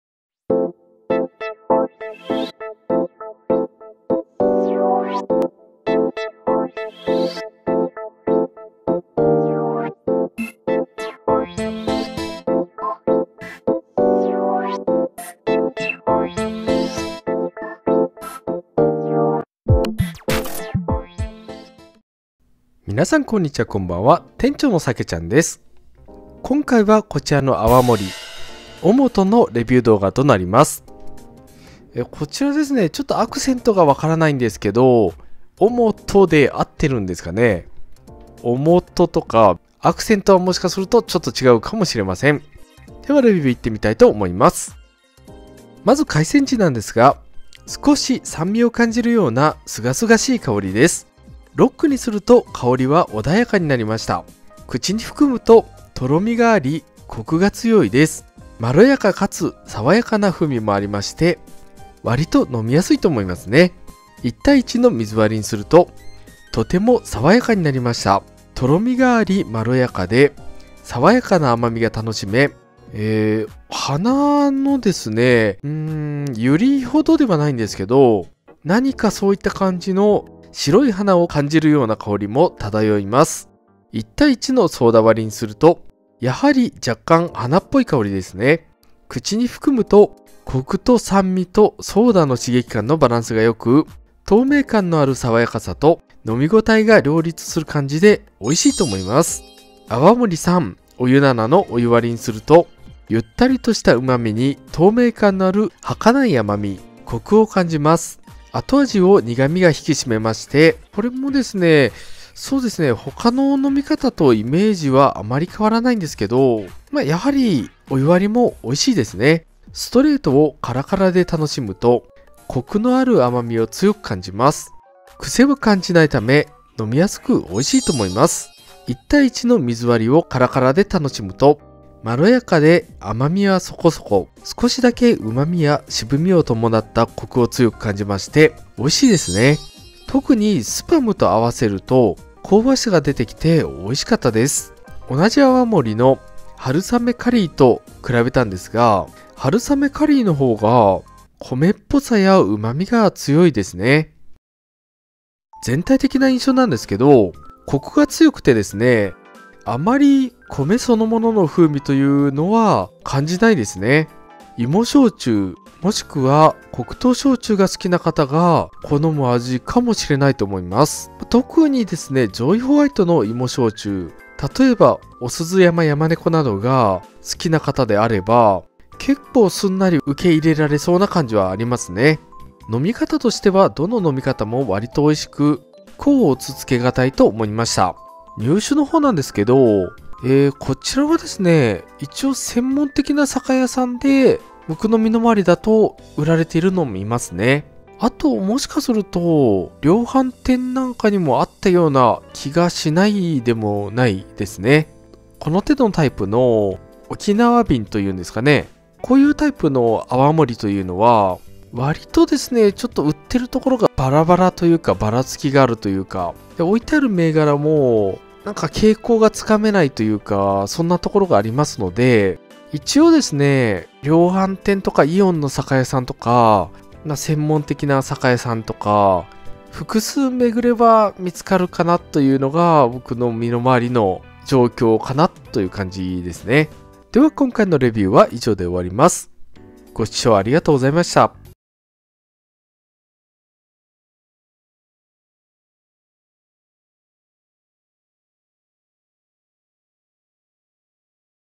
【泡盛】於茂登をレビューしてみました 色んな飲み方試しました
泡盛、於茂登をレビューしてみました。
飲みやすくてコクもあり、美味しいと思います。
カラカラ（沖縄の酒器）も登場しますが、注ぎ方は下手クソです。